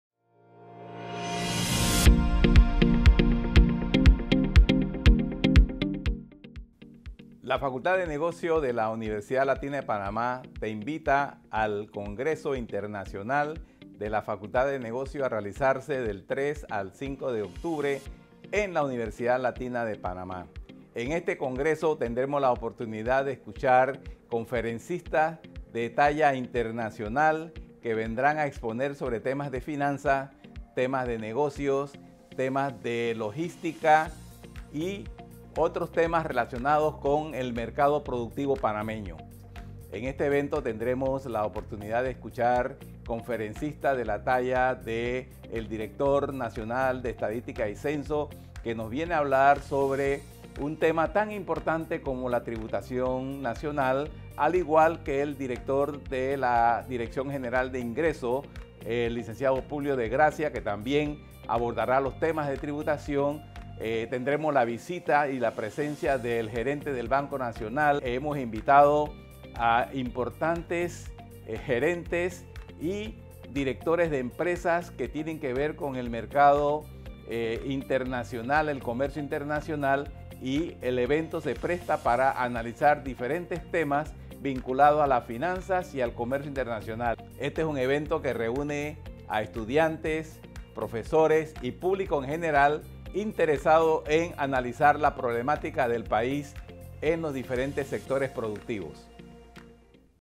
ULatina en los Negocios - Business for today: Congreso del Hub Financiero
Este congreso tiene un enfoque especial y relevante: "Hub Financiero y Logístico para las Américas". No solo exploraremos las tendencias y oportunidades en estas áreas críticas, sino que también crearemos un espacio donde las ideas innovadoras convergen y donde las conexiones se fortalecen.

Únete a nosotros del 3 al 5 de octubre para el Congreso Internacional de la Facultad de Negocios. Juntos, exploraremos el futuro de los negocios en las Américas.
Linkedin: Universidad Latina de Panamá